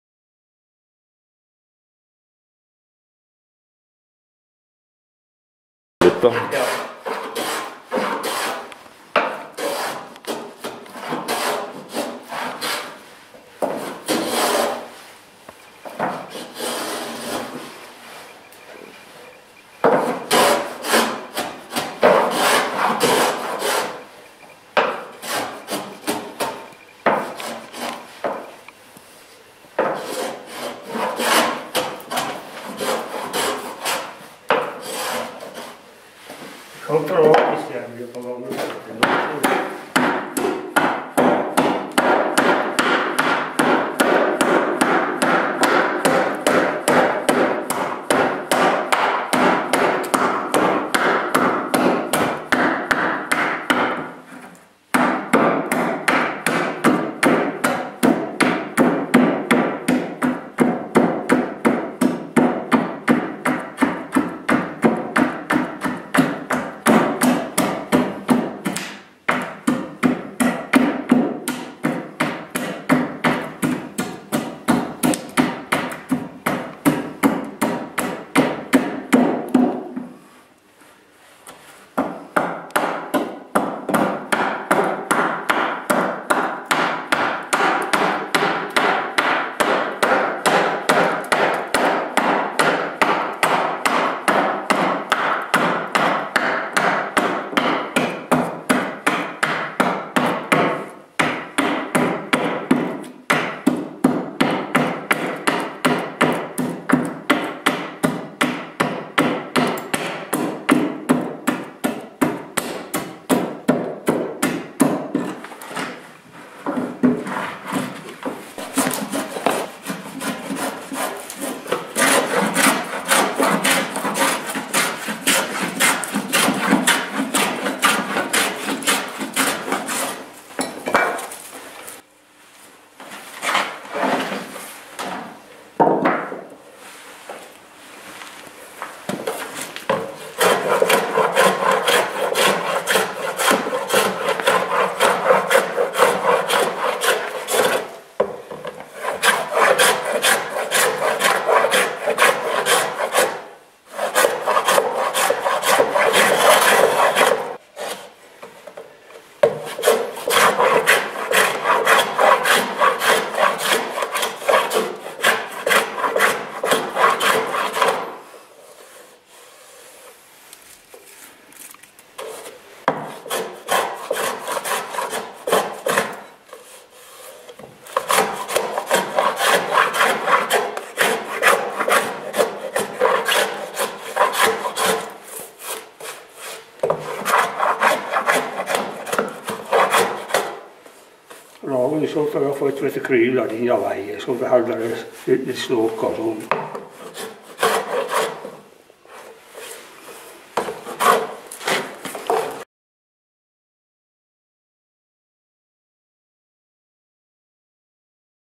Båtbygging i Bjørkedalen
Hogging og høvling av bordskøyt, ei skjerving. Båtbyggar er Jakob Helset.